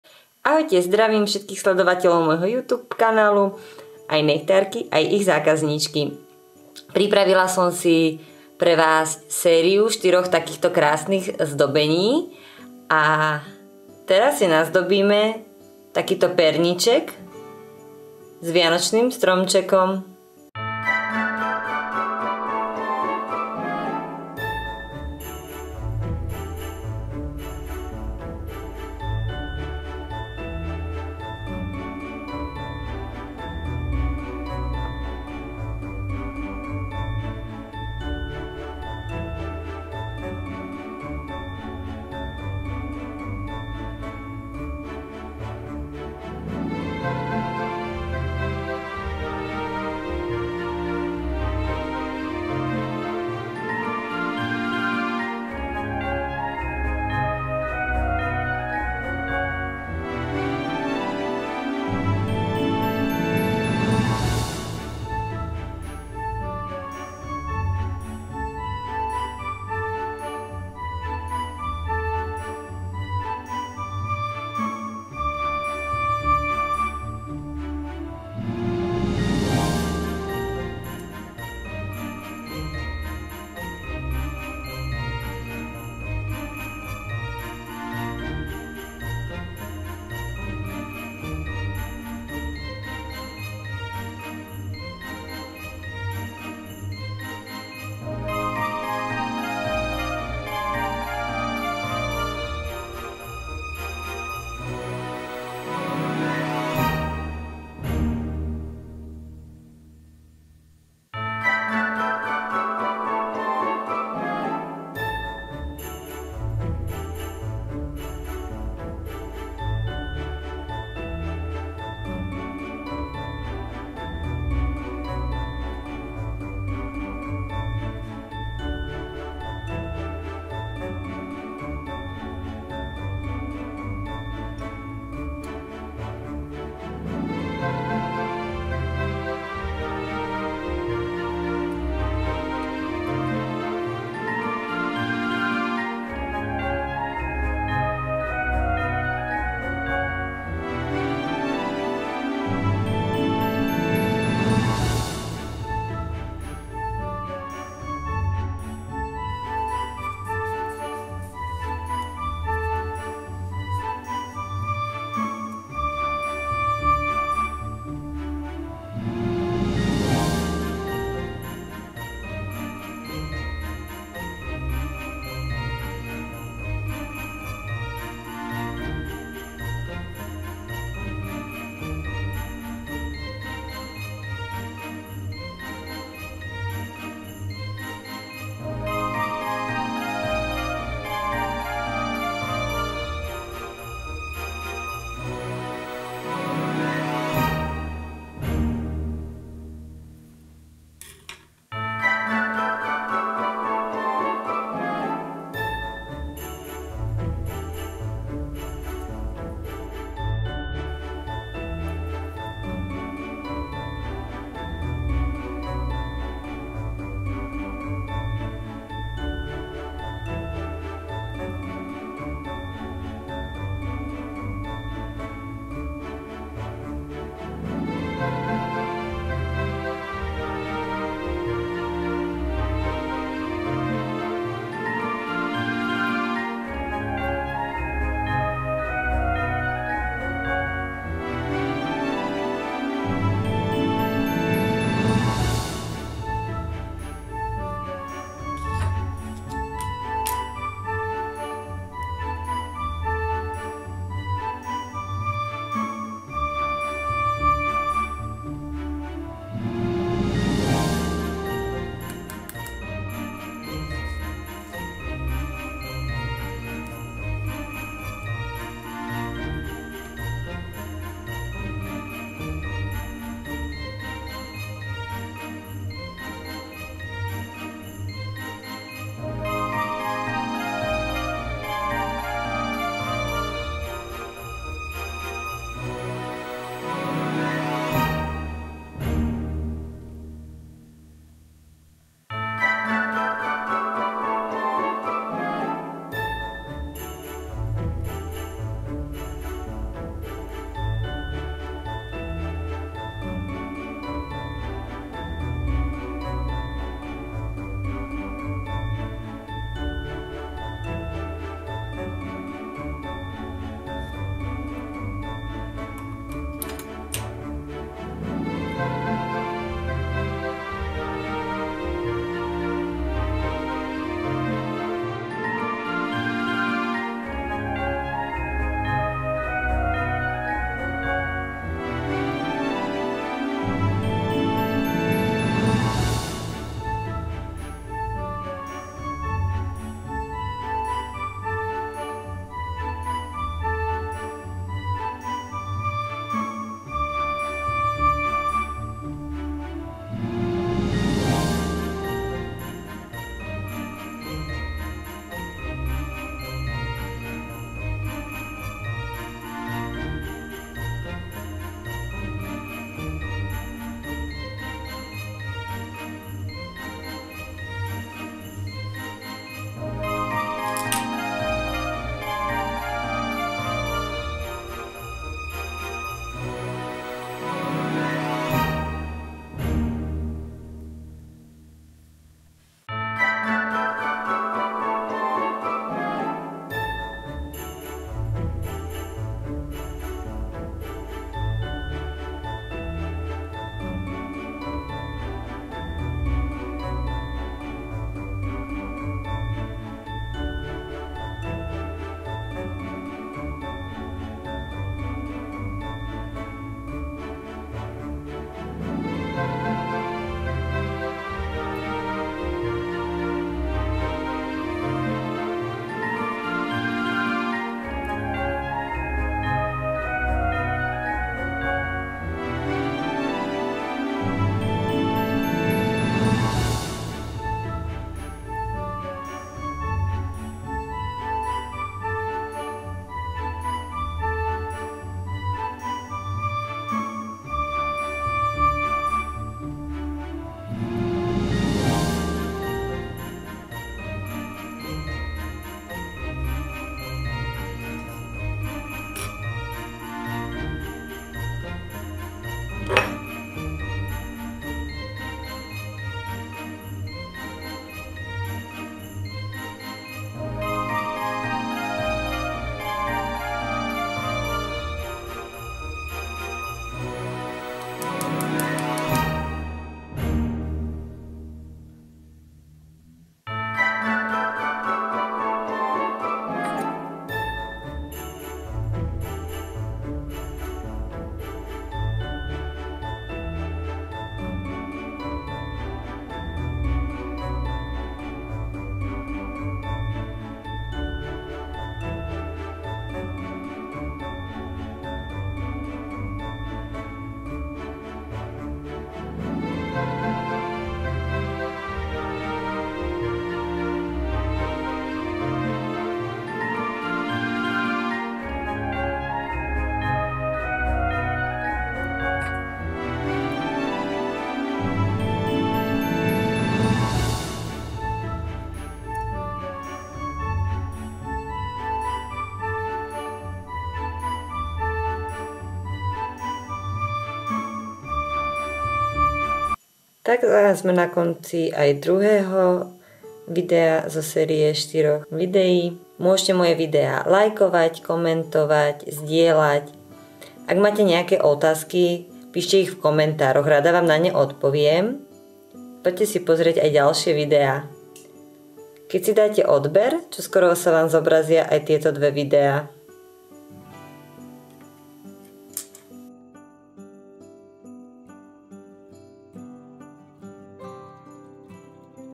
#10 Vianočné zdobenie: Perníček Stromček
Čaute nechtárky,
Pripravila som si pre vás sériu 4 vianočných Perníčkov, ktoré sa k sebe krásne hodia. Pozri si aj ďalšie moje videa:

Obsah:
00:00 Úvod
00:23 Podklad na Perníček v tvare stromčeka
02:34 Matný top
02:38 Poleva na perníku (cukrový efekt)
07:50 Malé hviezdičky na dozdobenie
08:49 Záver

Sociálne siete:
Facebook.com/nechty.katka